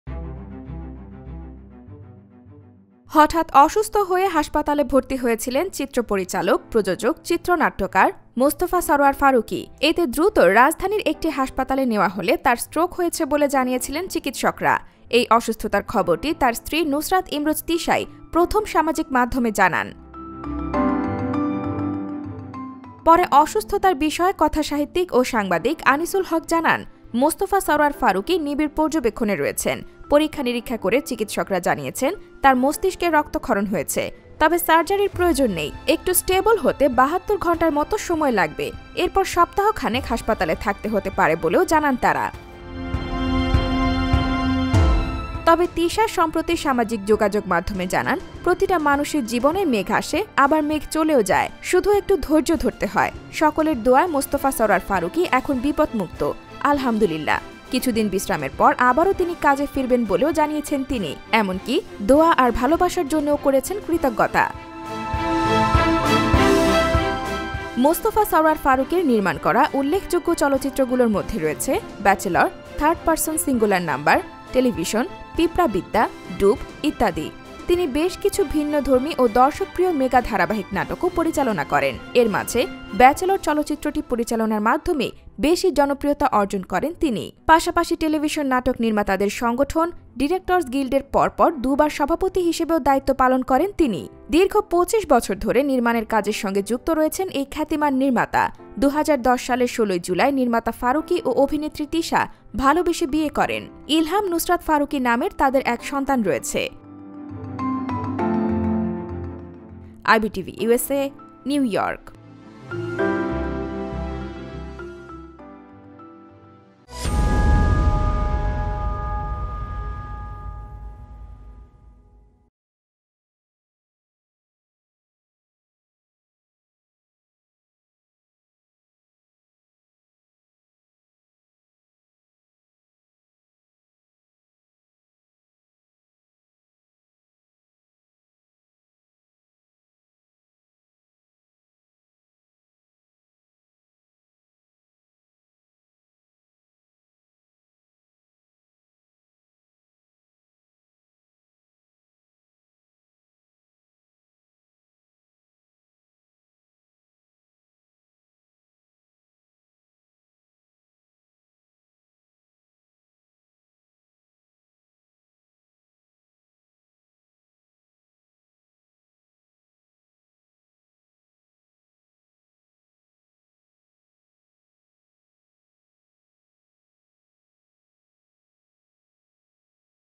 মোস্তফা ফারুকী এখন শঙ্কামুক্ত I Mustafa Faruqi I now safe I Tisha said I
Mustafa Faruqi is safe now
হঠাৎ অসুস্থ হয়ে হাসপাতালে ভর্তি হয়েছিলেন চলচ্চিত্র পরিচালক, প্রযোজক, চিত্রনাট্যকার মোস্তফা সরয়ার ফারুকী। এতে দ্রুত  রাজধানীর একটি হাসপাতালে নেয়া হহলে তাঁর স্ট্রোক হয়েছে বলে জানিয়েছিলেন  চিকিৎসকেরা ।এই  অসুস্থতার  খবরটি  তার স্ত্রী নুসরাত ইমরোজ তিশাই প্রথম সামাজিক মাধ্যমে জানান । পরে অসুস্থতার বিষিয়ে  কথাসাহিত্যিক ও সাংবাদিক আনিসুল হক জানান , মোস্তফা সরয়ার ফারুকী নিবিড় পর্যবেক্ষণে রয়েছেন। পরীক্ষা-নিরীক্ষা করে চিকিৎসকরা  জানিয়েছেন, তার মস্তিষ্কে রক্তক্ষরণ হয়েছে। তবে সার্জারির প্রযোজন নেই। একটু স্টেবল হতে ৭২ ঘন্টার মত  সময় লাগবে। এরপর সপ্তাহখানেক হাসপাতালে থাকতে হতে পারে বলেও  জানান তারা  ।